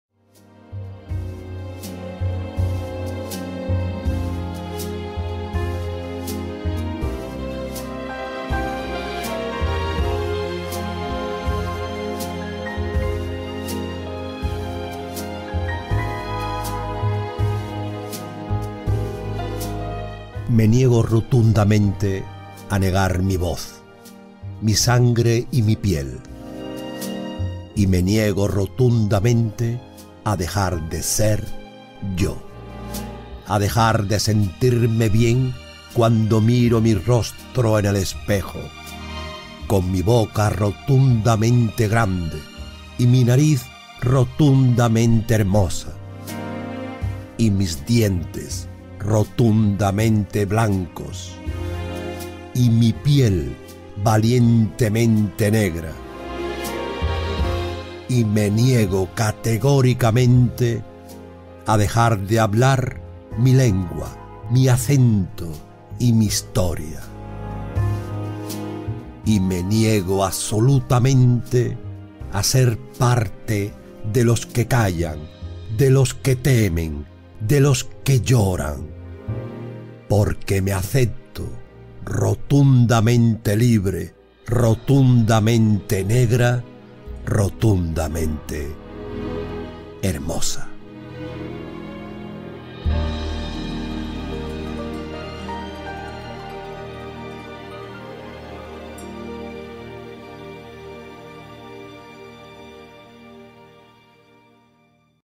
SHIRLEY CAMPBELL - Rotundamente negra.
Rotundamente negra

Me niego rotundamente
A negar mi voz,
Mi sangre y mi piel.
Y me niego rotundamente
A dejar de ser yo,
A dejar de sentirme bien
Cuando miro mi rostro en el espejo
Con mi boca
Rotundamente grande,
Y mi nariz
Rotundamente hermosa,
Y mis dientes
Rotundamente blancos,
Y mi piel valientemente negra.
Y me niego categóricamente
A dejar de hablar
Mi lengua, mi acento y mi historia.
Y me niego absolutamente
A ser parte de los que callan,
De los que temen,
De los que lloran.
Porque me acepto
Rotundamente libre,
Rotundamente negra,
Rotundamente hermosa.

Shirley Campbell